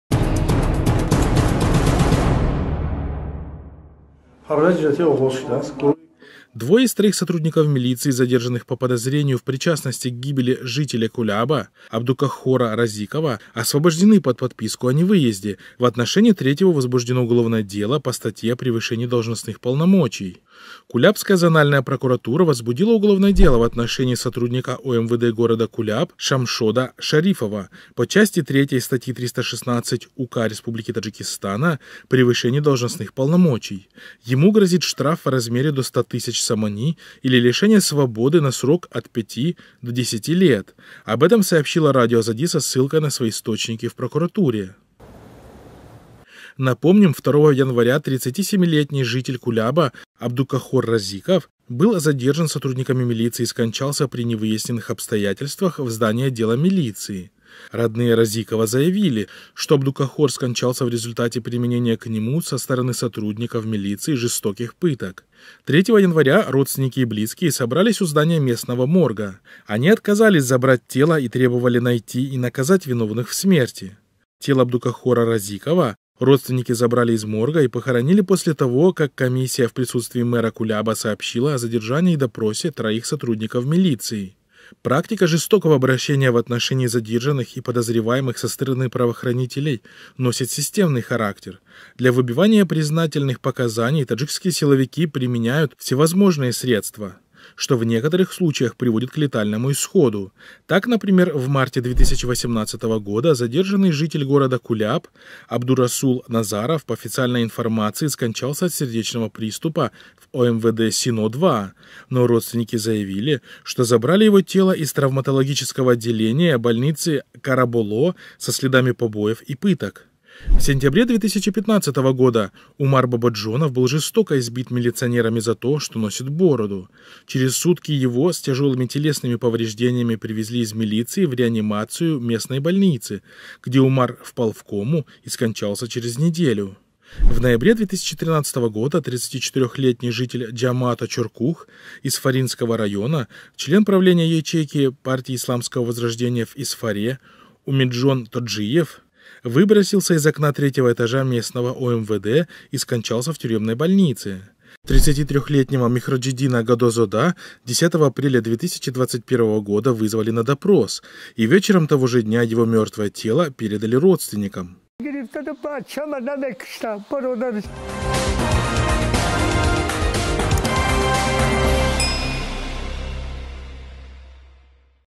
Куляб: милиционерам снова все сойдет с рук
Двое из троих сотрудников милиции, задержанных по подозрению в причастности к гибели жителя Куляба Абдукаххора Розикова, освобождены под подписку о невыезде, в отношении третьего возбуждено уголовное дело по статье о превышении должностных полномочий.